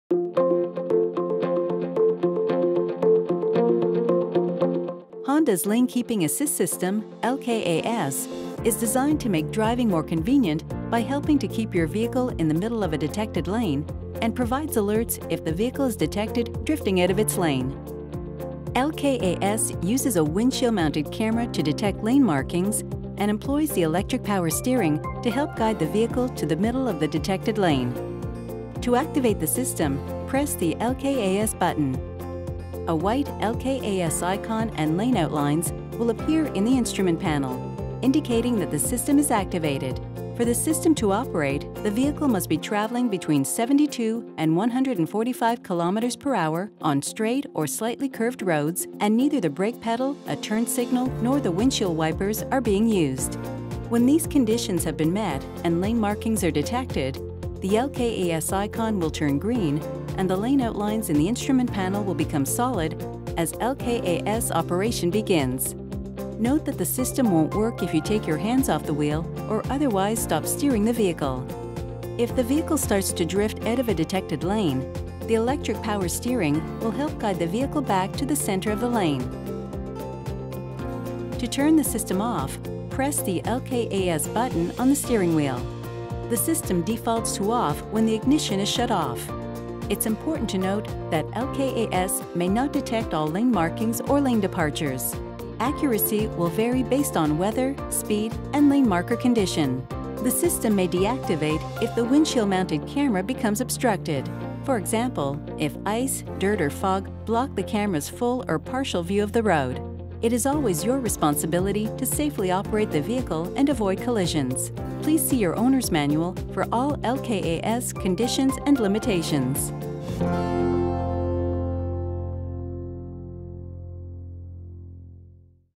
How to Use the Lane Keeping Assist System (LKAS)
Honda’s Lane Keeping Assist System—LKAS—is designed to make driving more convenient by helping to keep your vehicle in the middle of a detected lane, and provides alerts if the vehicle is detected drifting out of its lane.